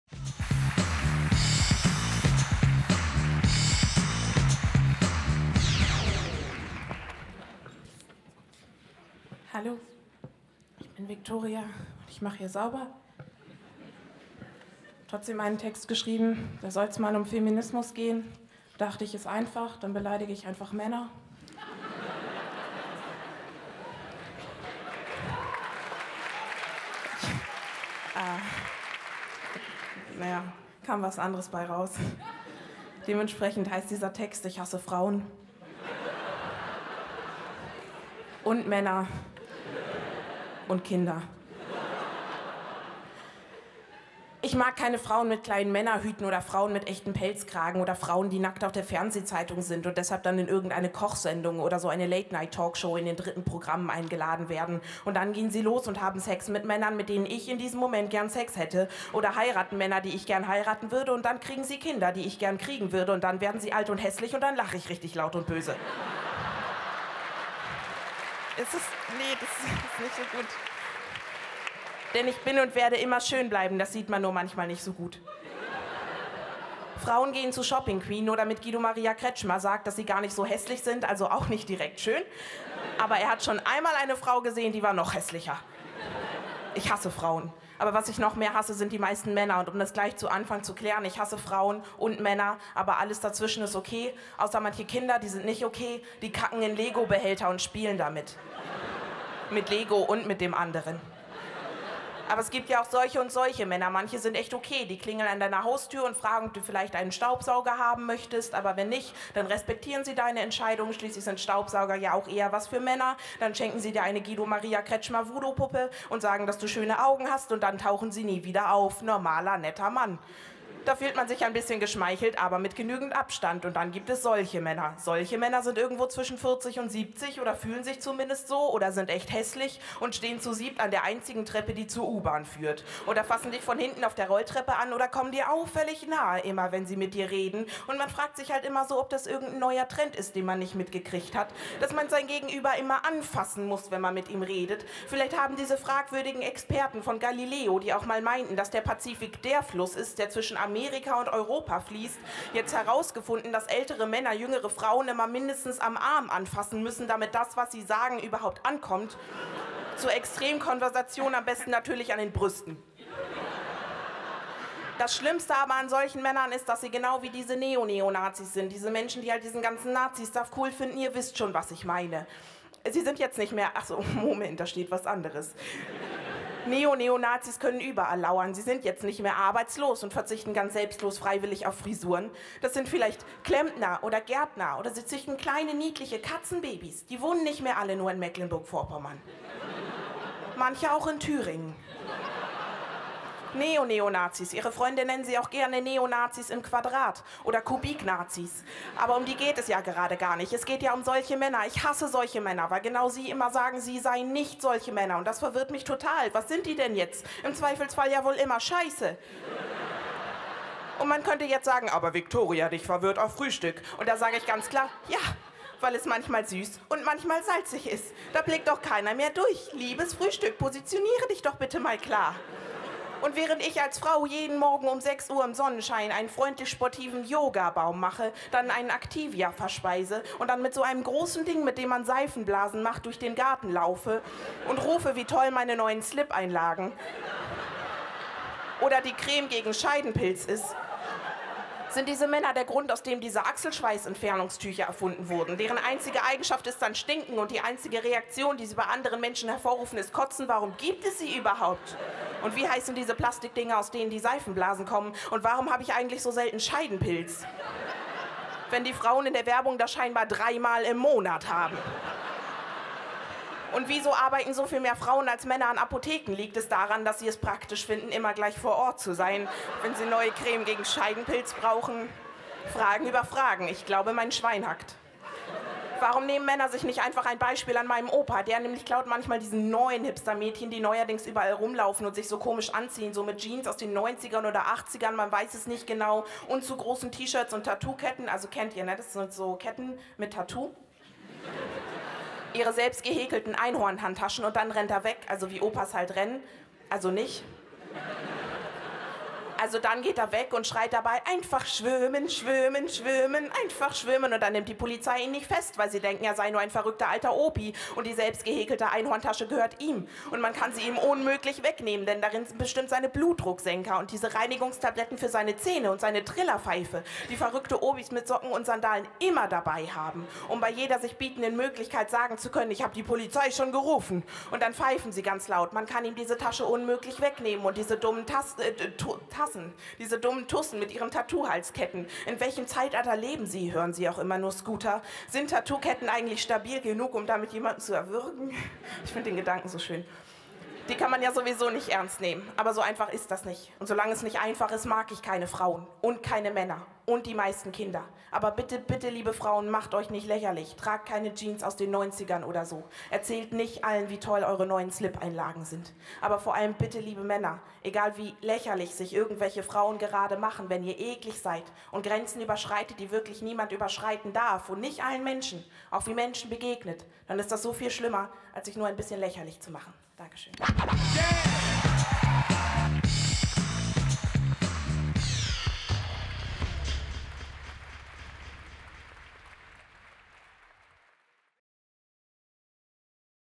Victoria Helene Bergemann - Ich hasse Frauen und Männer und Kinder
Victoria hat einen Text anlässlich des Frauentages geschrieben. Wer jetzt einen Feminismus-Rundumschlag erwartet, sollte dieses Video nicht anschauen. Victoria hasst Frauen. Und Männer. Und Kinder..."die kacken in Legobehälter und spielen damit". Viel Spaß!